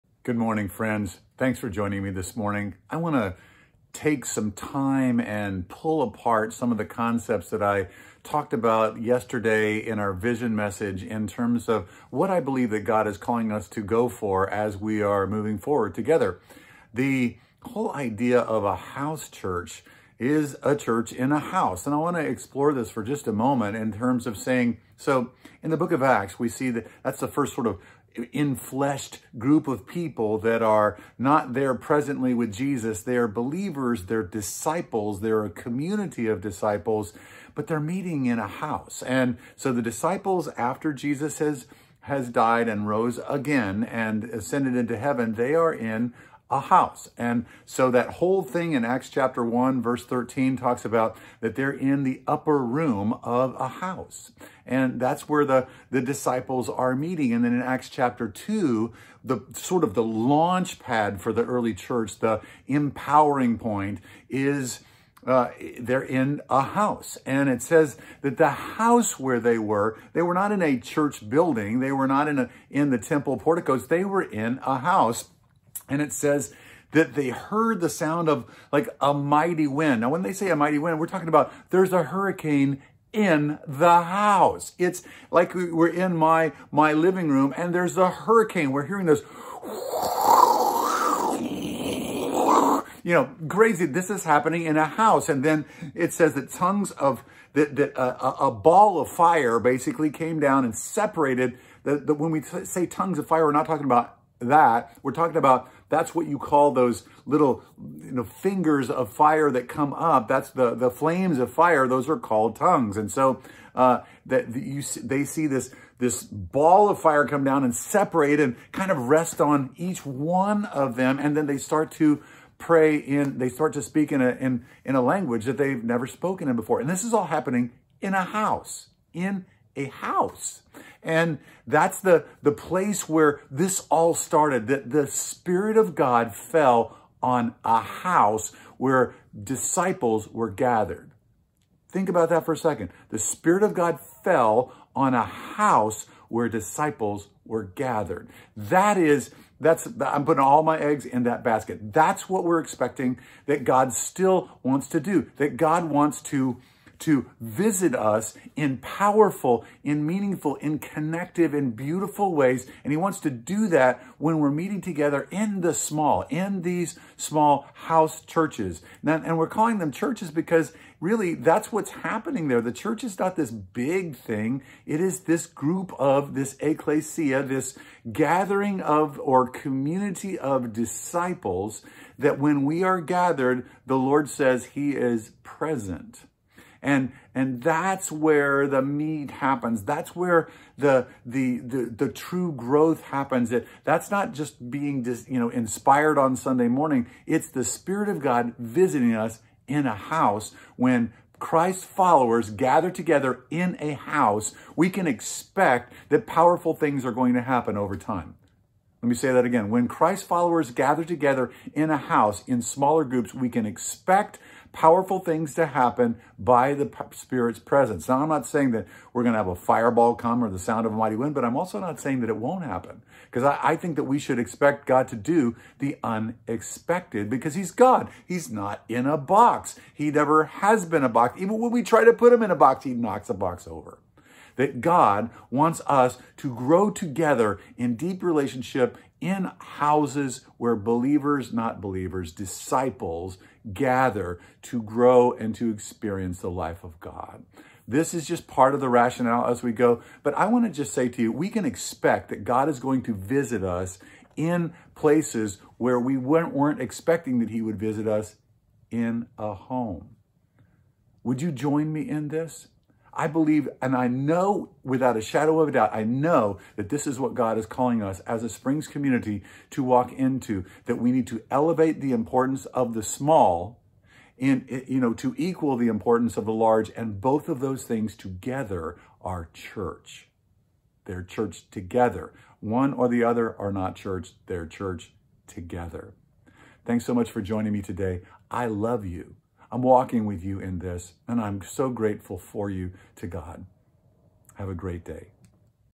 Powerful Things Happen In A House
The Spirit worked came and worked powerfully in a house--not a church building--and we can expect the same. (Acts 1:13; 2:2; 2:46; 5:42)